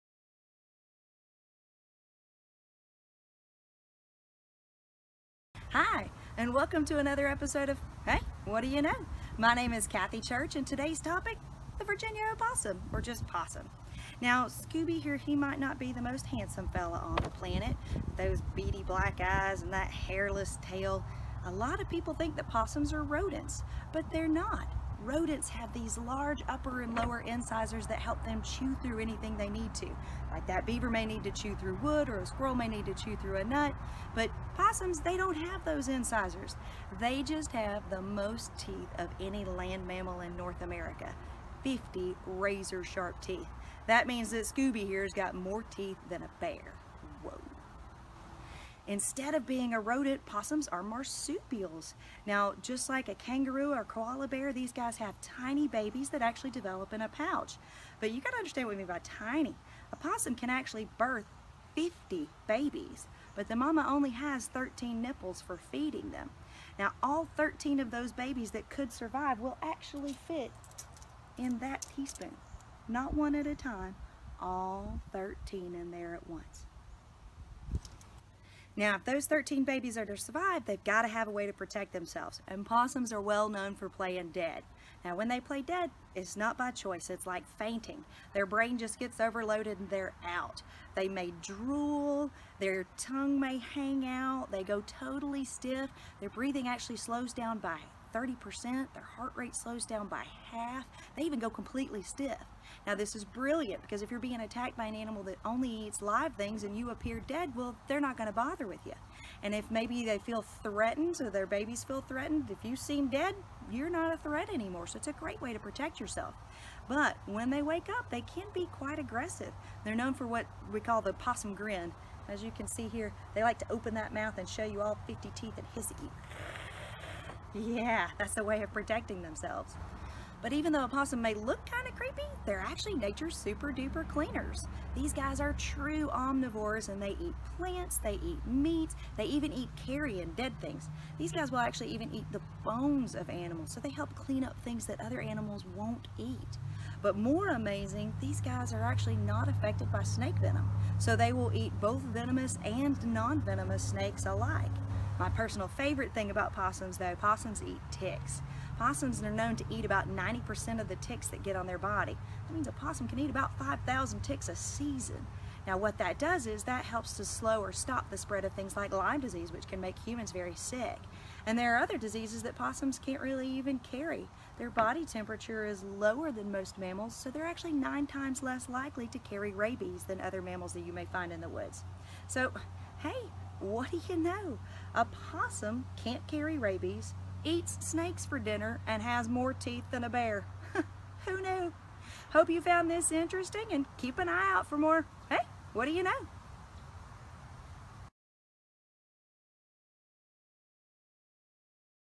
Hey, Whaddaya Know?!: Possums
Agh! Is that a rat? Nope, just an opossum. Join Programming Coordinator, Kathy Church, to find out all the amazing ways possums benefit the environment. and you!